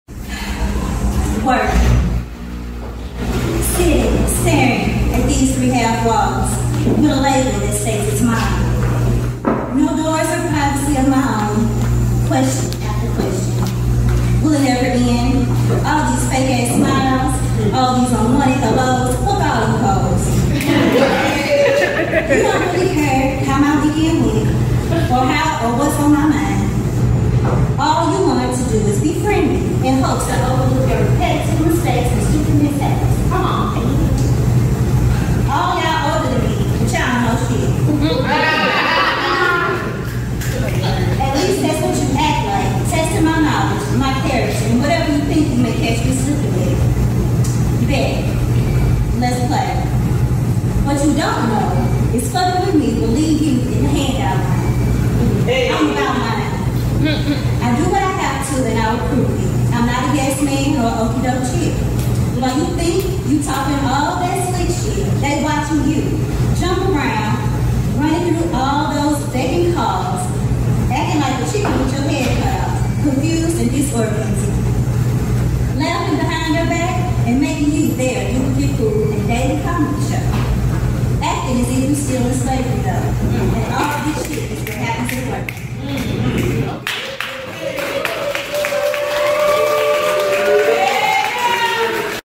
"Work" a poem written by Kindred
Kindred does a spoken word poem titled, "Work"